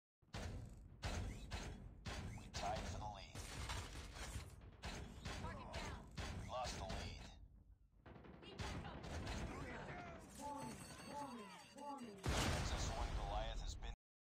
Trapped In The Place? LOL
I used XS1 Goliath and got trapped in the place😂 lol